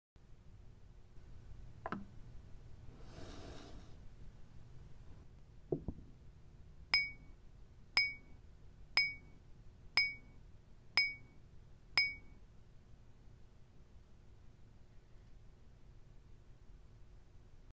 Umbra Prisma geometrischer Bilderrahmen
Prisma ist ein moderner Bilderrahmen im geometrischen Design.
Der Rahmen erzeugt einen schwebenden Effekt, indem er Ihr Lieblingsfoto zwischen zwei Glasscheiben schweben lässt. Das geometrische Design des Rahmens ist durch die Vorderseite des Rahmens hindurch sichtbar und erzeugt so eine gewisse Dimension und Tiefe. Der Fotorahmen Prisma ist in verschiedenen Ausführungen und Größen erhältlich und ist vertikal sowie horizontal als Wand- und Tischbilderrahmen einsetzbar. Der Rahmen lässt sich problemlos in jeden Einrichtungsstil integrieren und ist deshalb auch ein beliebtes Geschenk und Mitbringsel.